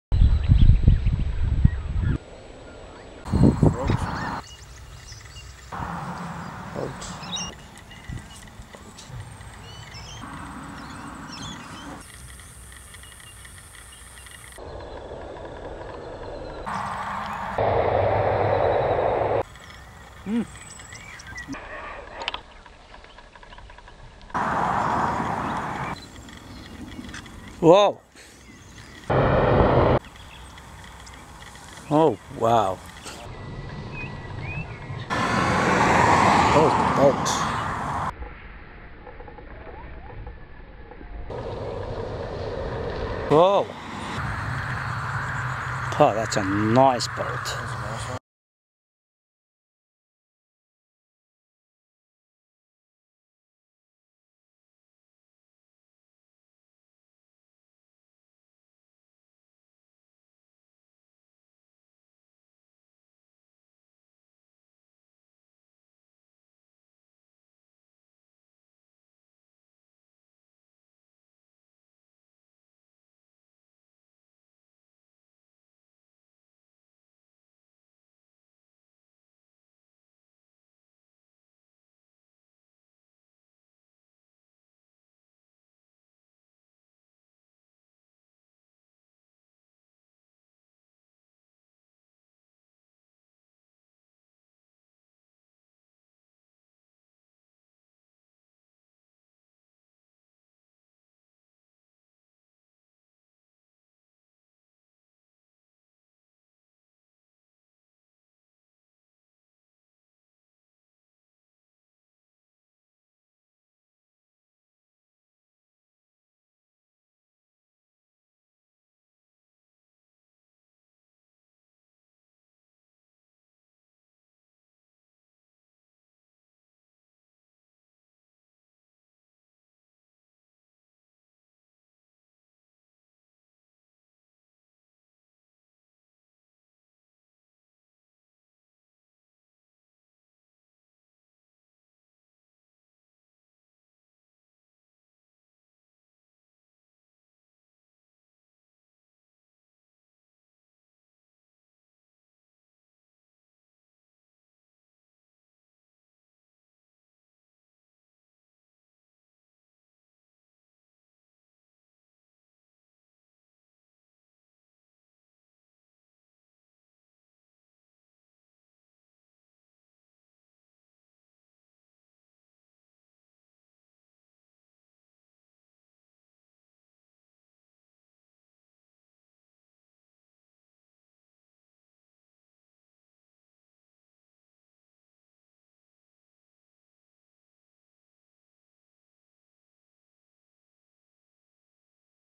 Storm Lightning Mangrove Mountain 1st November 2015
Spectacular lightning storm approached Mangrove Mountain. Timelapse and lightning with incredible scenery!